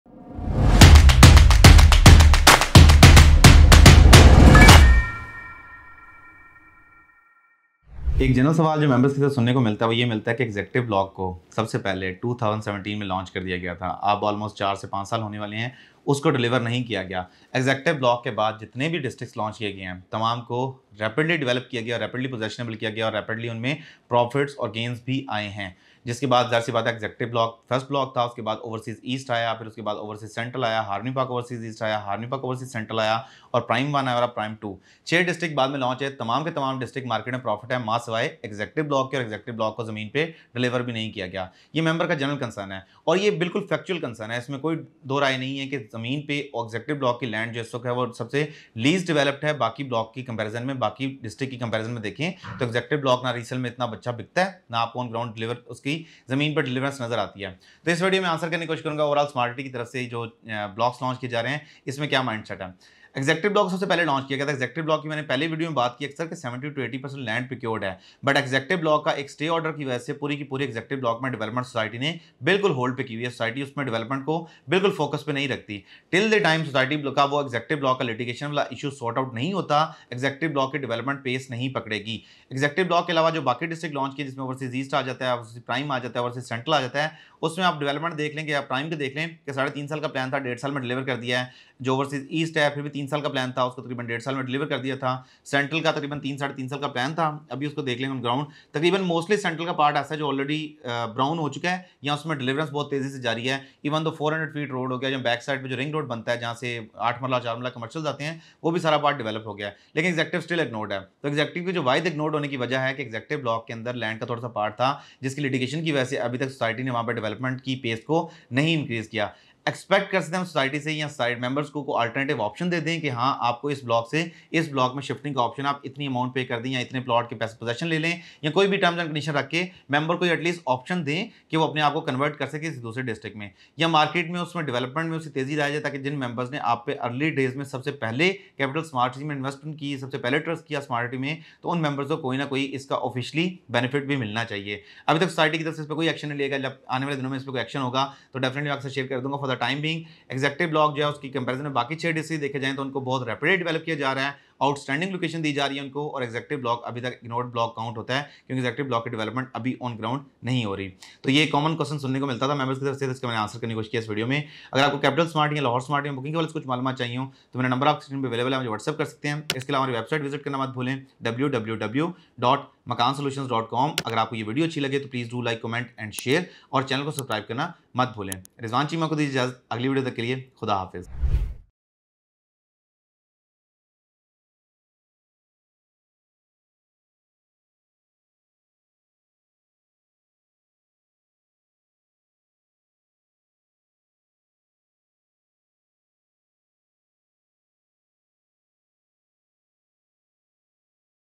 Why has Capital Smart City not delivered Executive Block yet even launched in 2017? Reasons of Delay
All other six districts which are launched after the Executive Block are delivered too. The development work in the Executive Block is delayed because of land state order. You will see a rapid development after getting rid of this legal problem. The Executive block in Capital Smart City is considered an ignored block because of no round work yet.